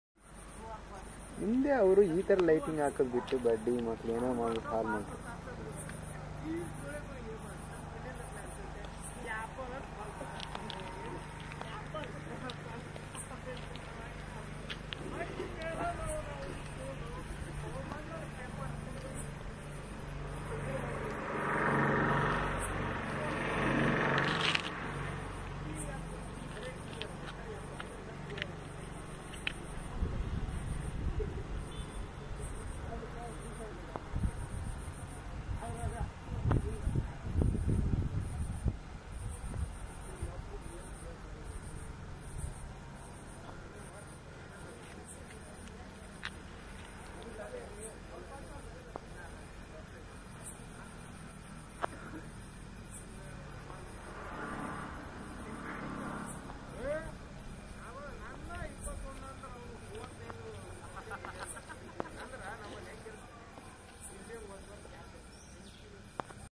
Davangere University
Davangere University campus is very beautiful infrastructure and buildings...